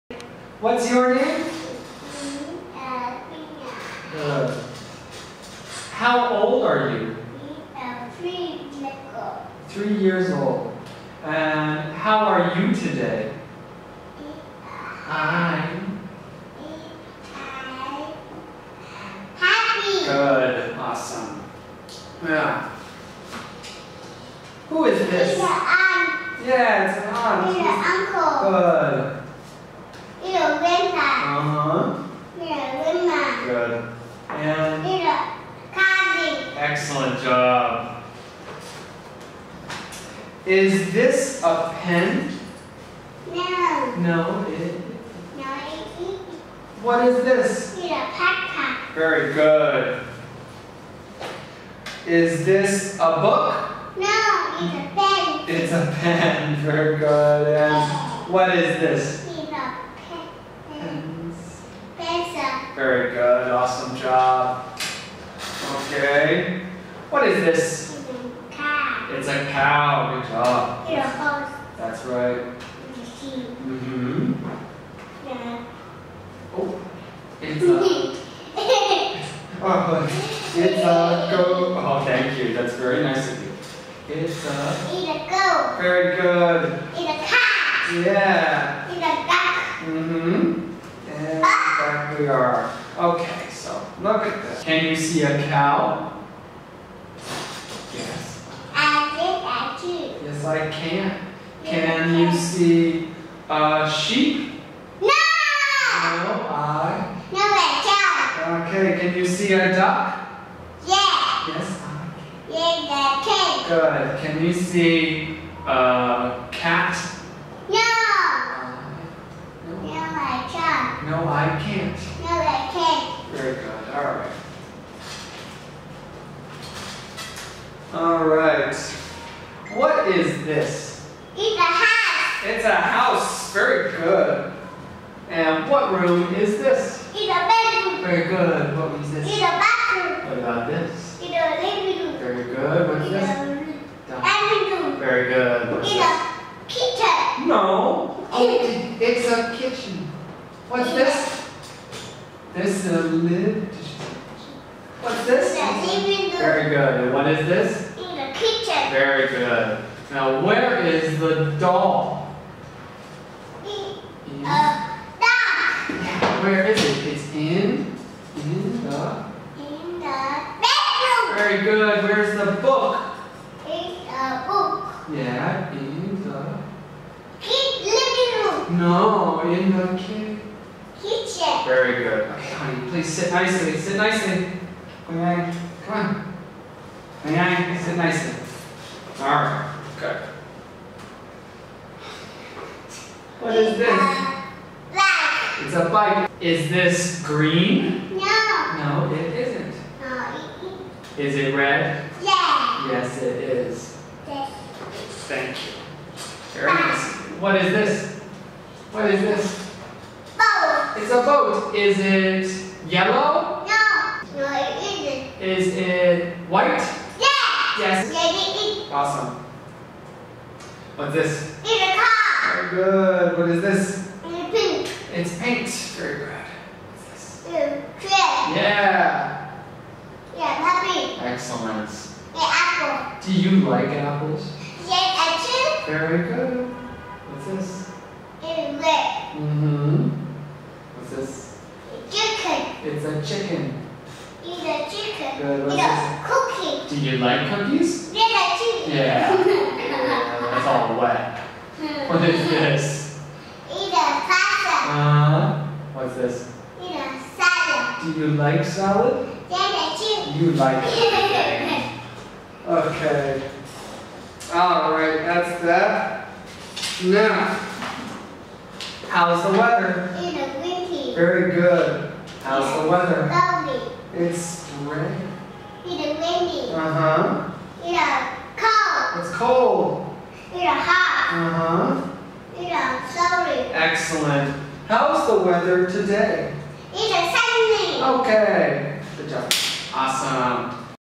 Bài Test cuối khóa - Quỳnh Anh
Quỳnh Anh is an absolute delight in class and always very involved and interested, resulting in a very strong grasp of the material, particularly vocabulary. I would like her to speak in full sentences more readily, and she can use some work on agreeing responses with question words like can and do. Her pronunciation, as to be expected of a student her age, is not perfect. Sometimes letter sounds get mixed up like in packpack (backpack) but she speaks very confidently which is very promising. I have no doubt that she will excel as she continues to learn english, and she will continue delighting everyone around her.